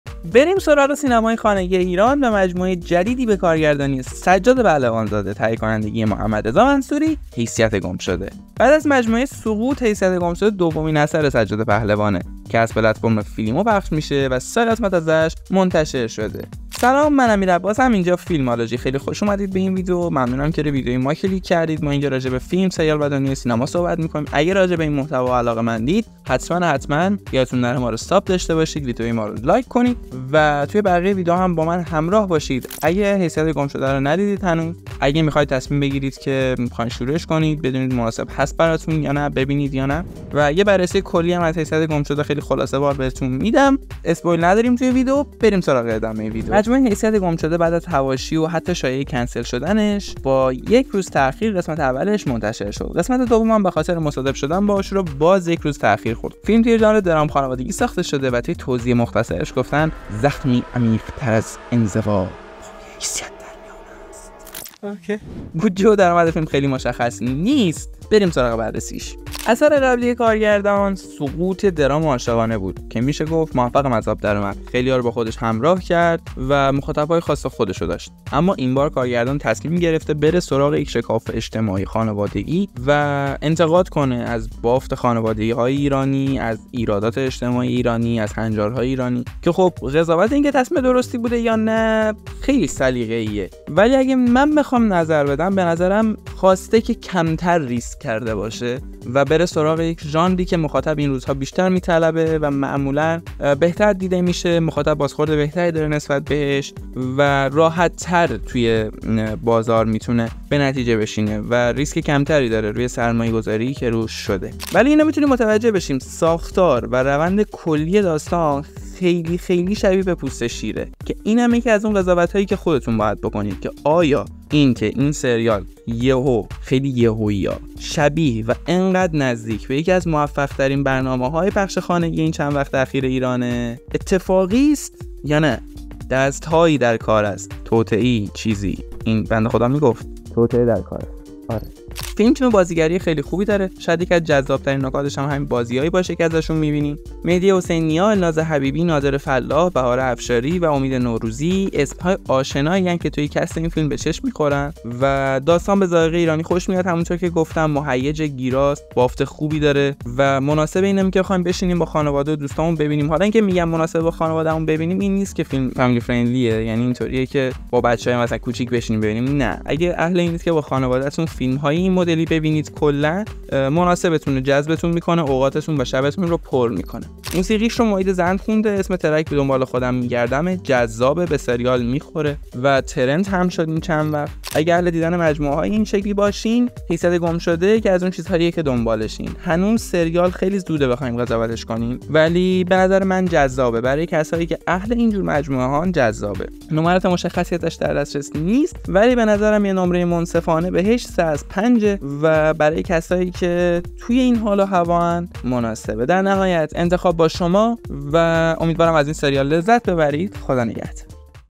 معرفی و موشکافی سریال ایرانی حیثیت گمشده | خیلی شبیه پوست شیر هست
امروز می خوایم بریم سراغی یکی دیگه از سریال های سنیما خانگی به کارگردانی سجاد پهلوان زاده و تهیه کنندگی محمدرضا منصوری.
حیثیت گمشده بعداز کلی حواشی و شایع های مختلف کنسل شدن بالاخره از پلتفرم فیلیمو تو ژانر درام، اجتماعی، خانوادگی پخش شد.
من تو این ویدئو علاوه بر معرفی و موشکافی سریال حیثیت گمشده بهتون میگم که ببینیمش یا نه.
پس تا آخر ویدئو همراهم باشین و اگه این سریال دیدین حتما نظرتون تو کامنت ها برام ارسال کنید